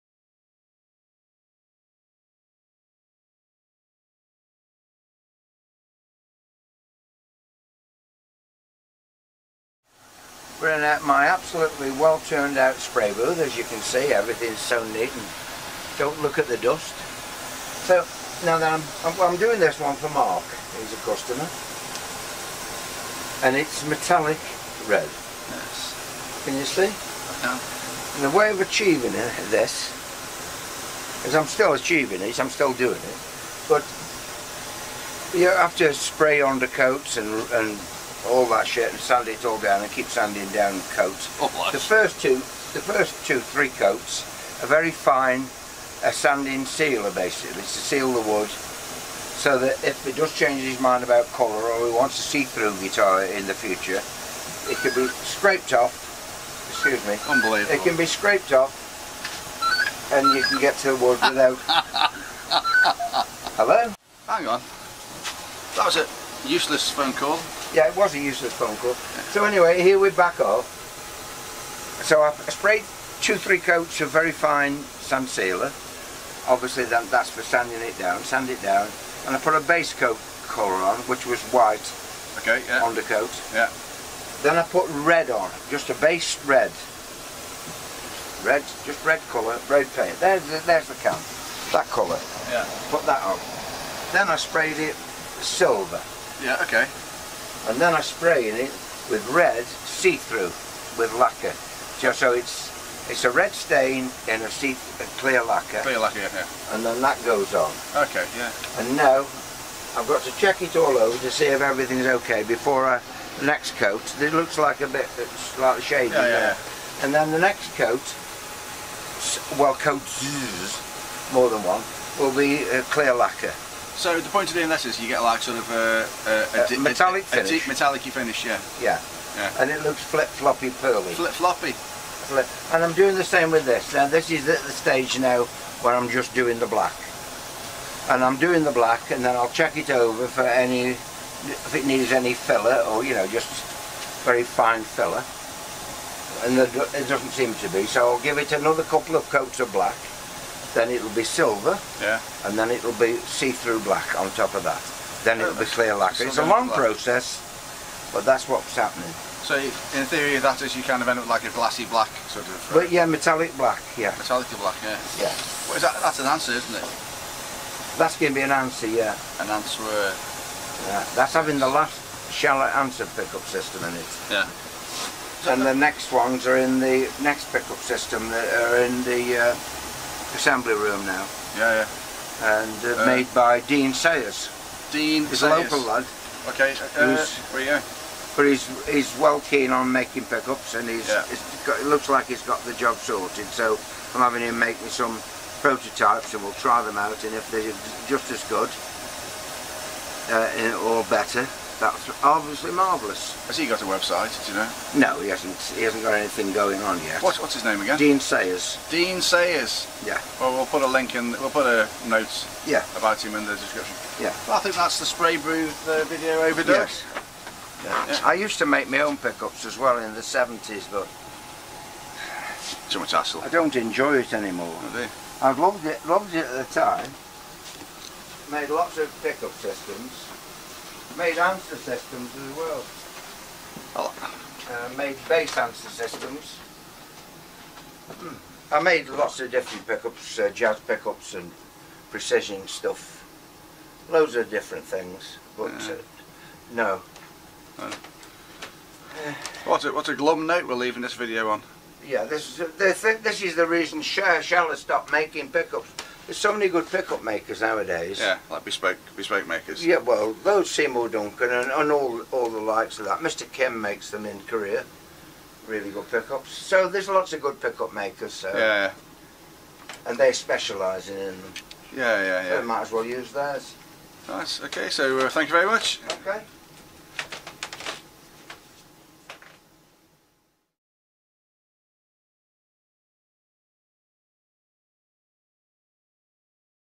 How to Spray a Metallic Paint Finish on an Electric Guitar - Doug Wilkes Workshop 23rd Feb 2023
Here Doug Wilkes explains the technique of building up various colours and lacquers to eventually achieve a specific deep metallic finish on an instrument.  It's a long process but the results are well worth the effort.

Doug also talks about his history of making his own pickups, and mentions Dean Sayers who has recently made some pickups for Doug's 'Answer' System.